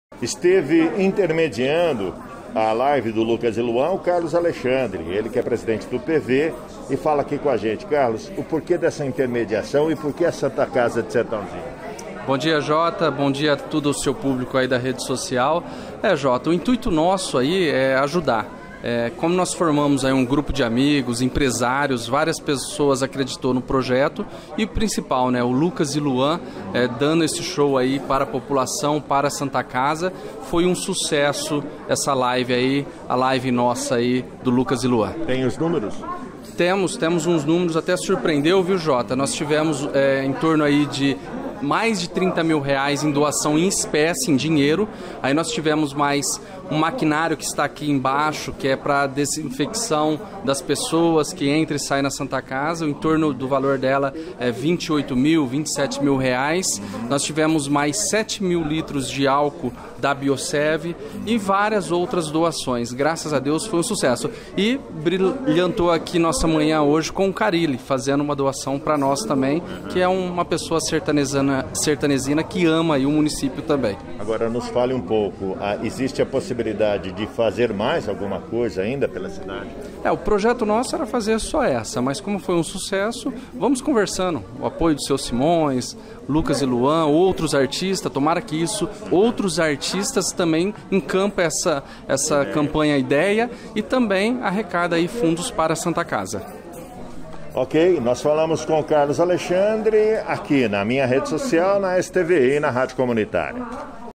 LIVE LUCAS E LUAN CONSEGUIU 25.000,00 PARA SANTA CASA - CARLOS ALEXANDRE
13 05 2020 - LIVE LUCAS E LUAN CONSEGUIU 25.000,00 PARA SANTA CASA - CARLOS ALEXANDRE